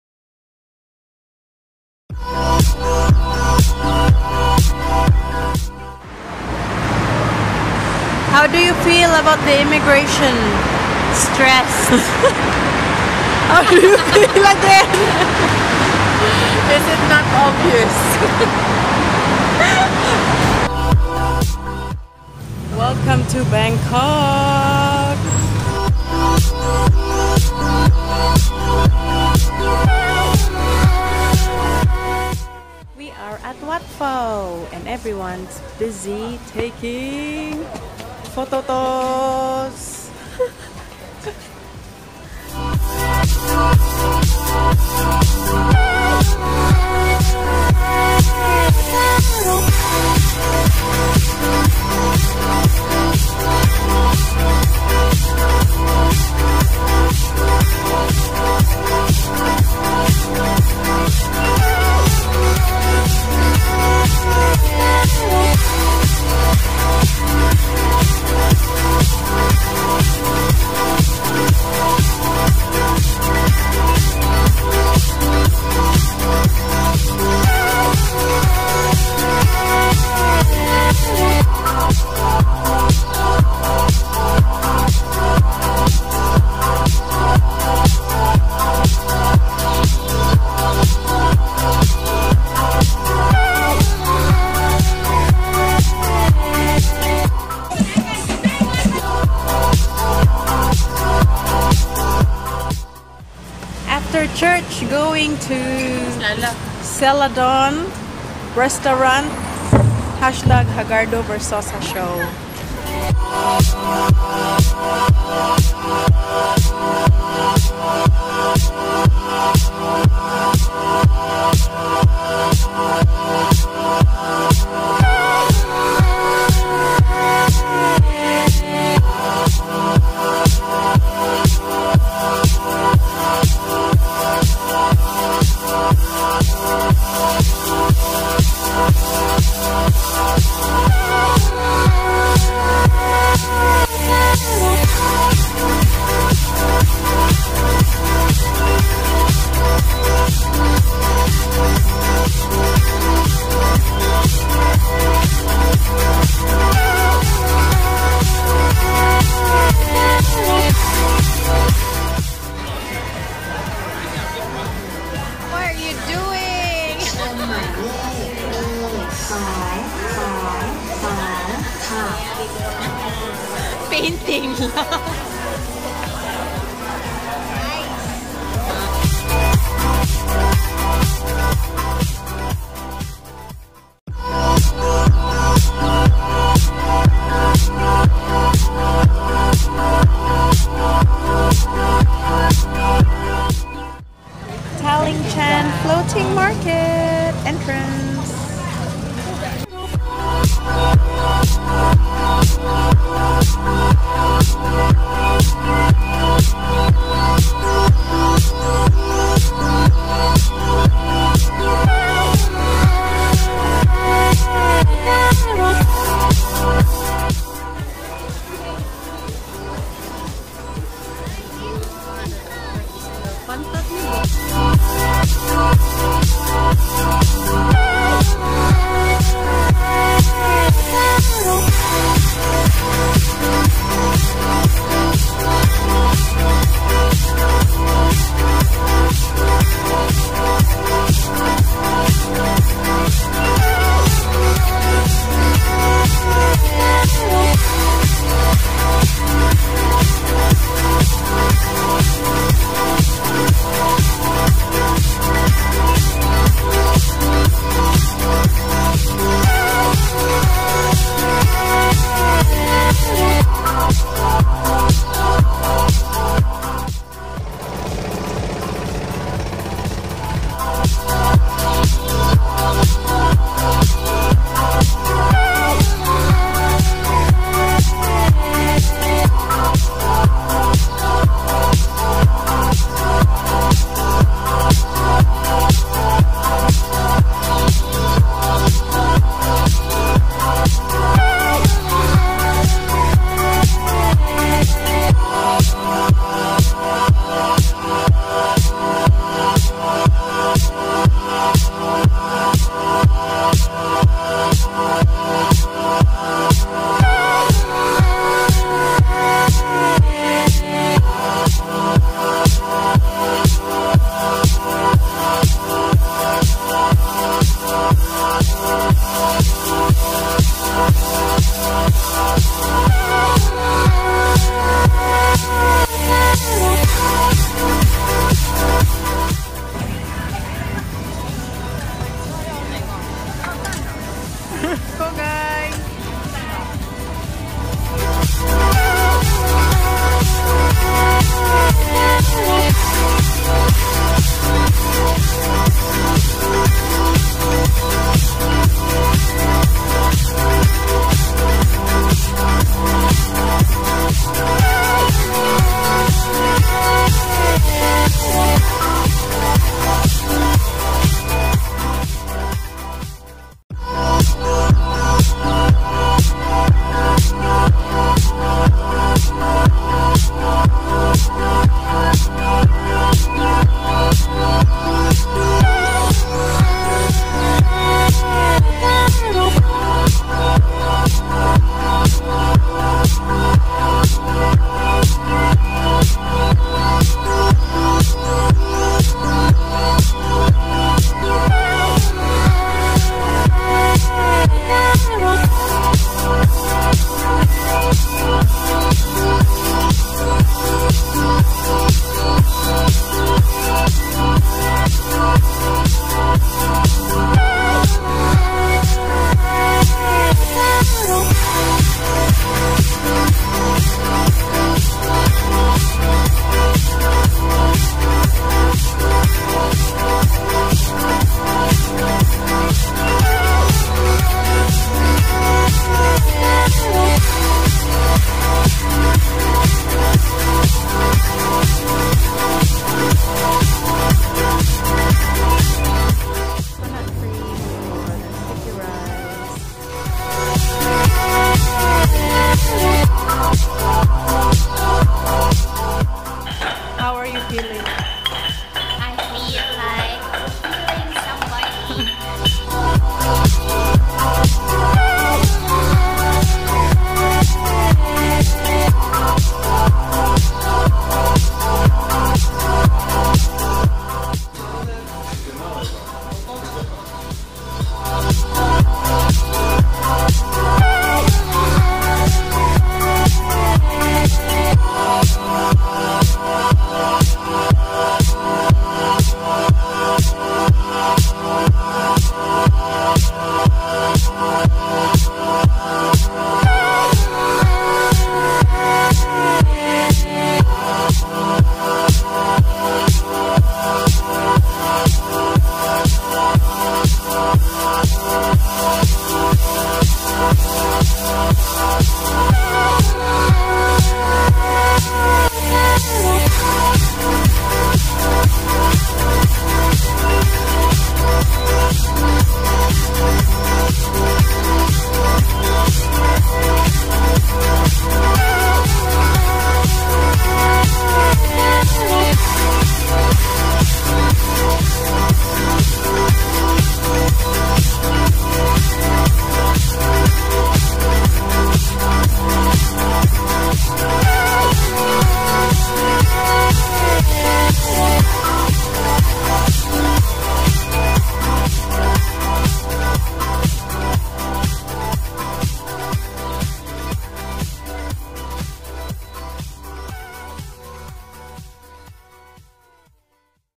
050818 - Ganzas in Bangkok
Full on days of touring, eating and shopping in Bangkok! An awesome place of temples with a modern urban city twist! A very clean city too. Avoided the tuktuks and took "Grab" cars/taxi everywhere when we were not on our feet. Danjah and I were the survivors of the heat and exhaustion. Haha! Had pad thai overload too! 😍 Didn't get to see the grand palace as the guards were too strict with the clothing restrictions. Might be a sign for me to have a reason to go back! 😉